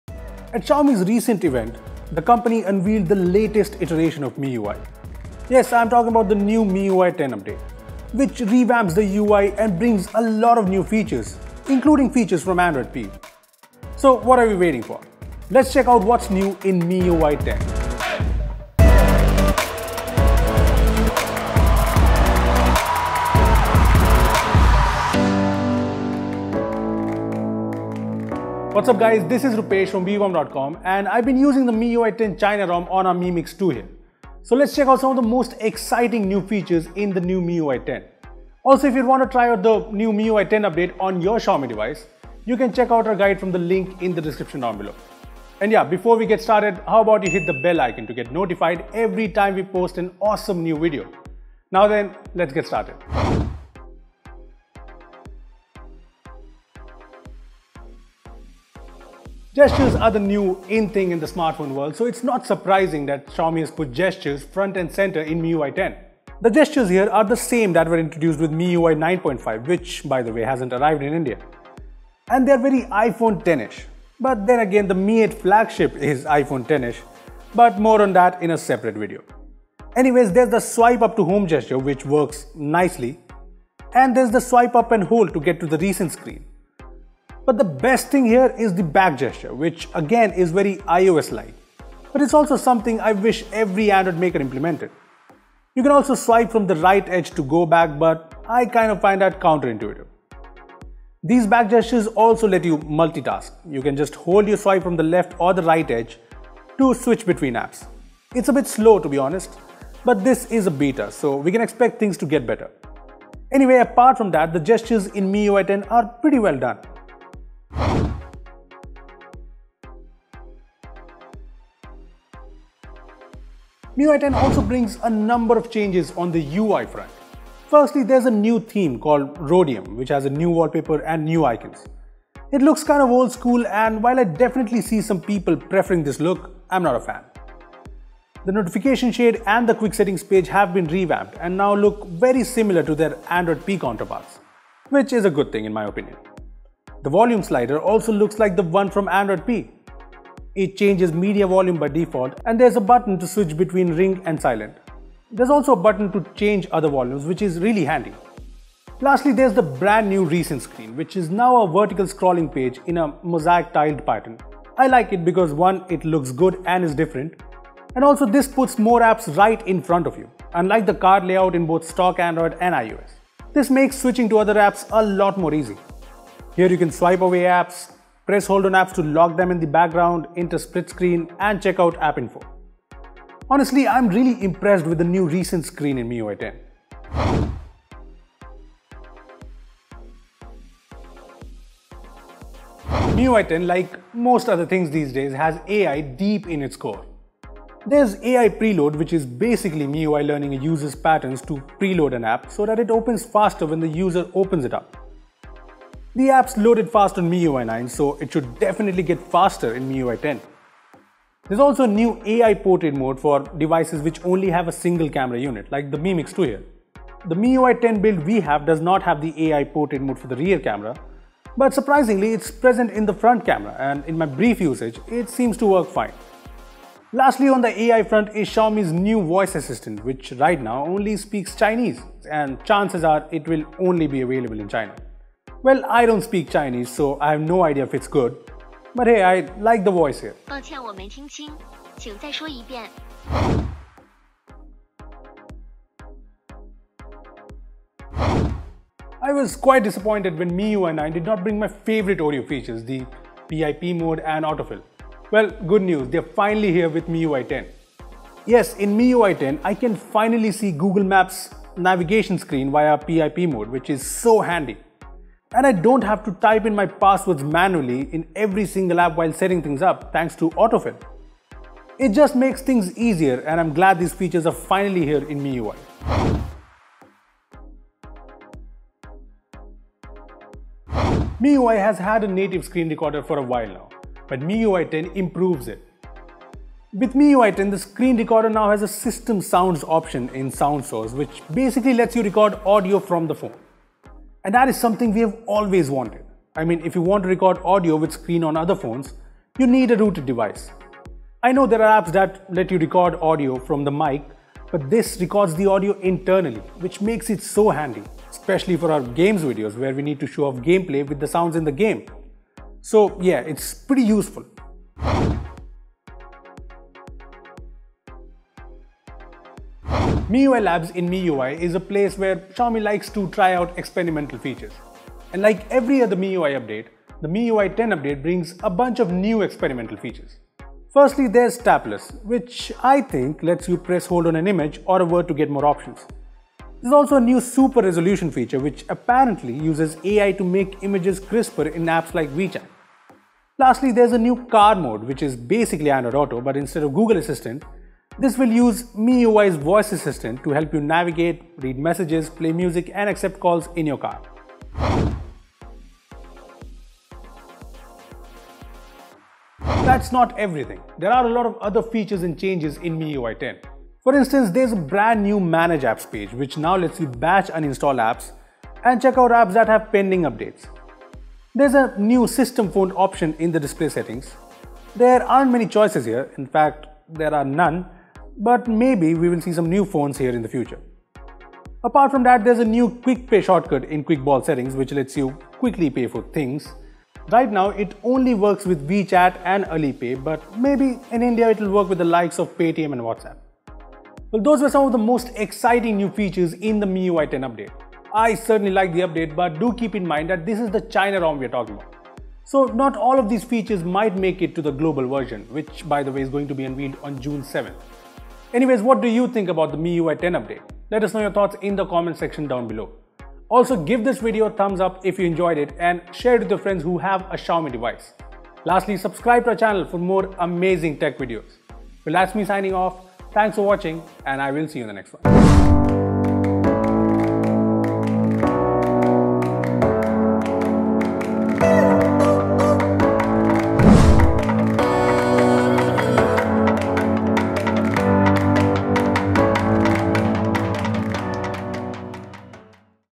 7 New MIUI 10 Features!
Xiaomi recently announced the MIUI 10 update for Redmi Note 5 Pro, Mi 6, Mi Mix 2, Mi Mix and other Redmi and Mi smartphones. Well, we have installed it on our Mi Mix 2 and here are the 7 most exciting and new MIUI 10 features we have found.

We are showing the new MIUI 10 features on the Mi Mix 2 and Mi 6.